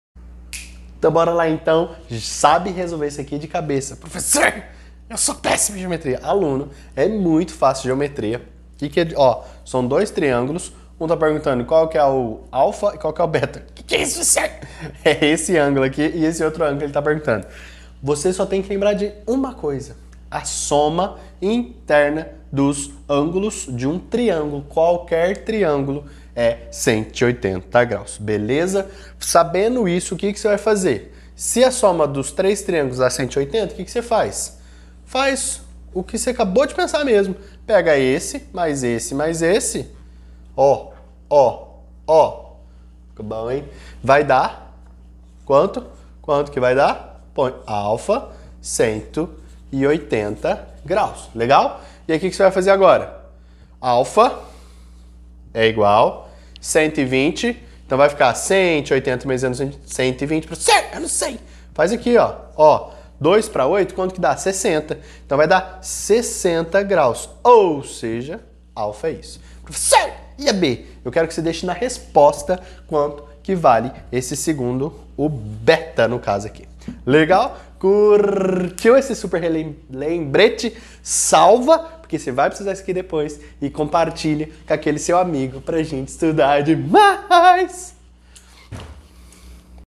🐸 Como calcular a soma dos ângulos internos sem enlouquecer? Aprenda!
🐸 Você sabia que a soma dos ângulos internos de um triângulo sempre será 180 graus? Essa é uma curiosidade muito interessante que envolve a geometria!

Muitas vezes nos deparamos com triângulos em nossas vidas, seja em cálculos matemáticos ou em construções. Saber que a soma de seus ângulos internos é sempre igual a 180 graus pode ser útil em várias situações.

Além disso, é importante destacar que essa regra também se aplica a outros polígonos regulares, como quadriláteros, pentágonos e hexágonos. Saber sobre a soma dos ângulos internos pode te ajudar em diversos momentos e te tornar um verdadeiro mestre da geometria!

E você, sabia dessa curiosidade? Deixe um comentário com sua opinião ou um emoji de sapinho 🐸 se achou interessante! Vamos estudar mais juntos? 😉